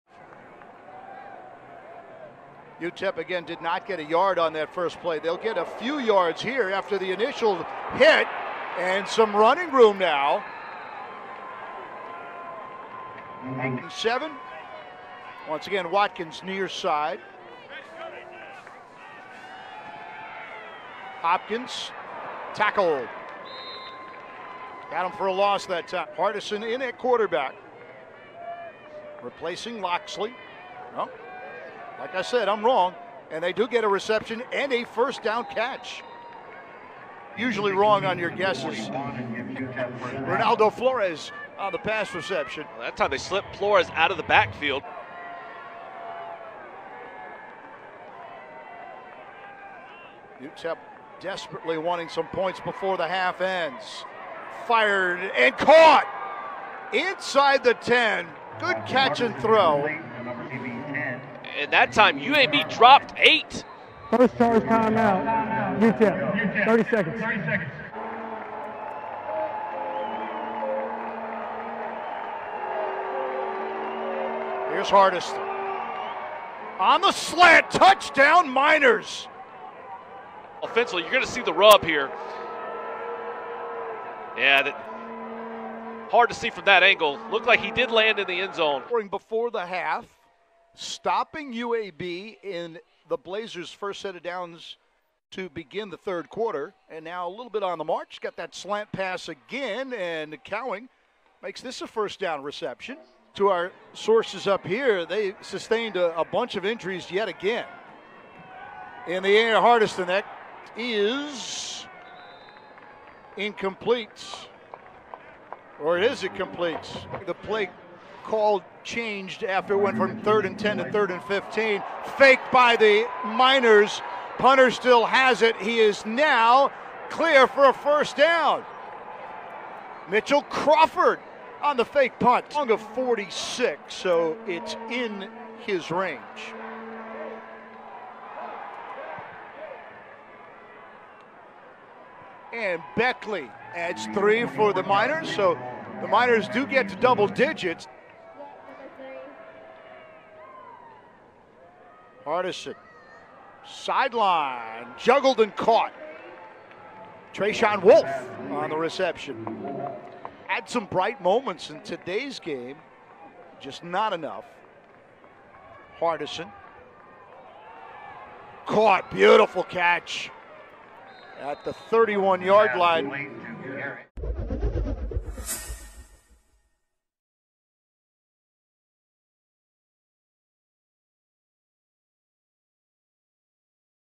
UTEP Football Top Plays vs. UAB (2019) | Stadium
UTEP QB Gavin Hardison passed for 222 yards and a touchdown, and WR Jacob Cowing hauled in five catches for 85 yards in the Miners' 37-10 loss to UAB.